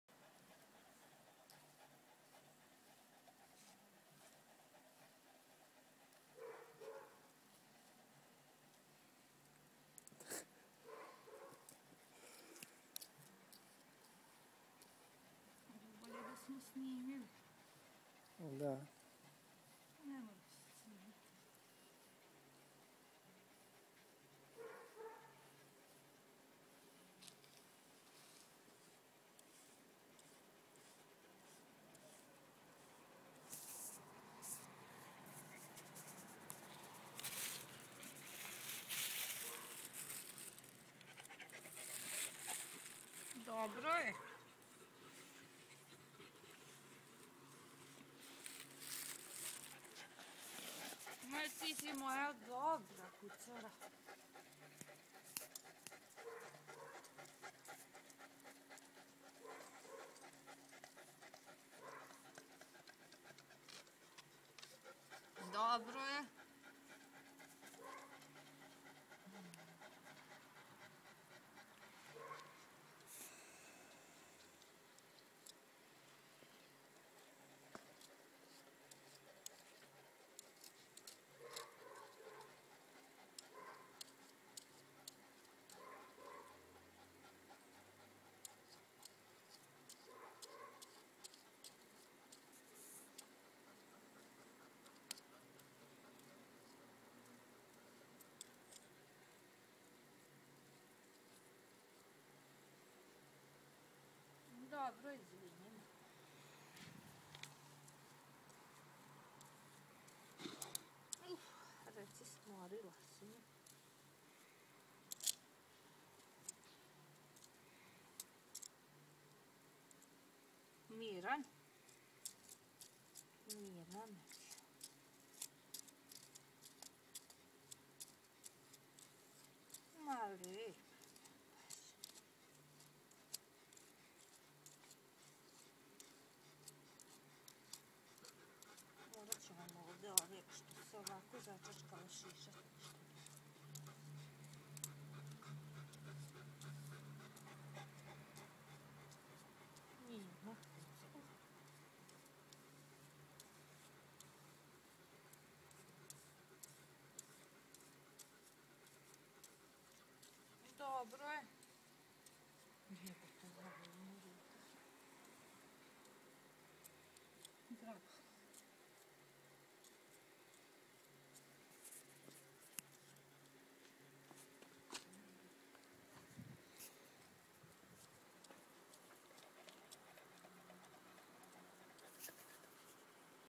Grooming our St. Bernard
Combing our 20 months old St. Bernard ...